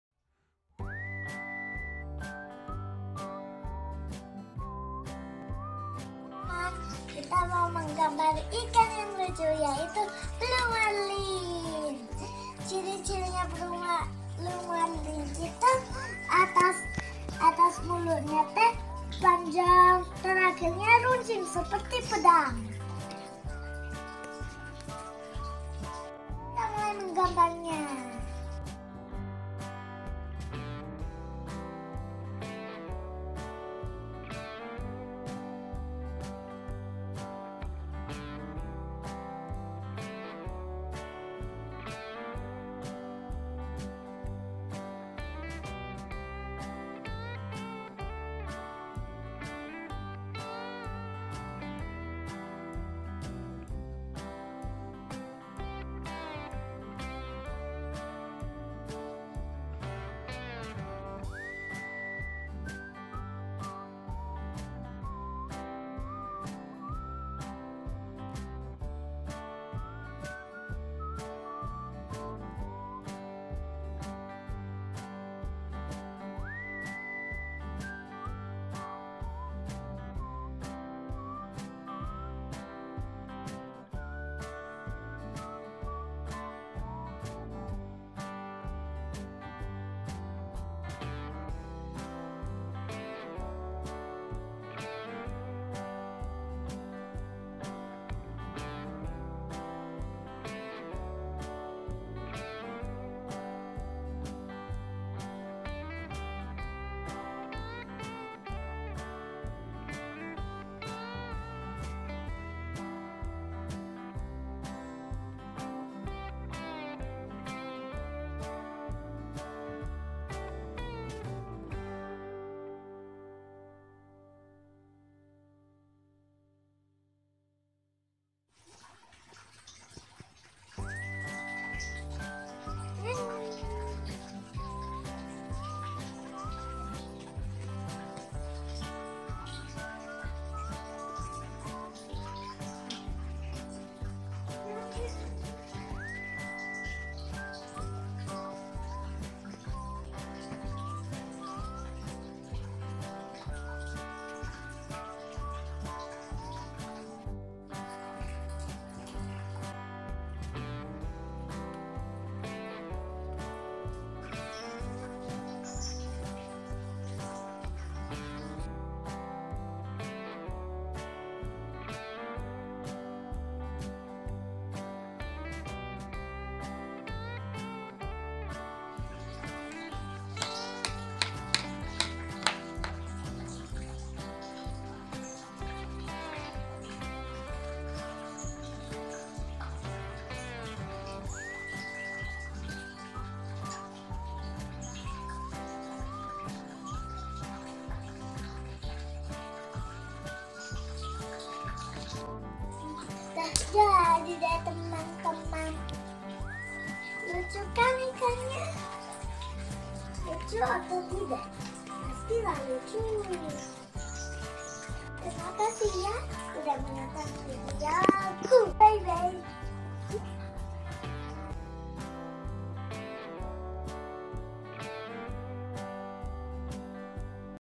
CARA MUDAH MENGGAMBAR BLUE MARLIN UNTUK ANAK2(EASY TO DRAW BLUE MARLIN FOR KIDS)
Video ini ini menunjukkan cara mudah menggambar ikan yang lucu yaitu blue marlin. Caranya sangat mudah diperagakan oleh anak TK.
Cobain yukk. gampang loh